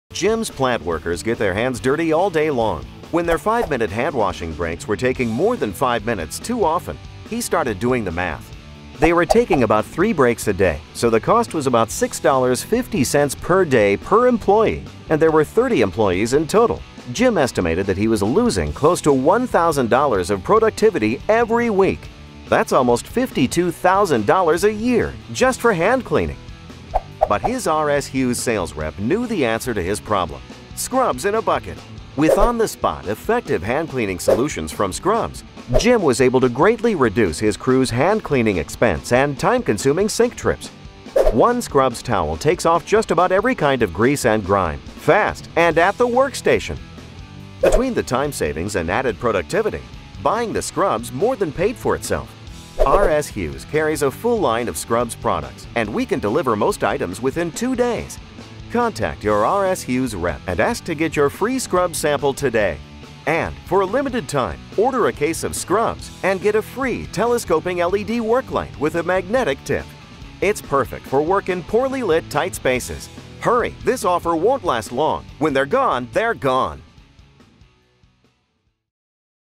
RS Hughes SCRUBS Video with Free Flashlight Promo Tag
Rivet|MRO developed this video sales promotion for R. S. Hughes and SCRUBS. The graphics animation video shows customers how they can save money by switching from cleaning hands with soap-and-water to SCRUBS. It also features a 15-second promo tag with a limited-time FREE telescoping flashlight offer.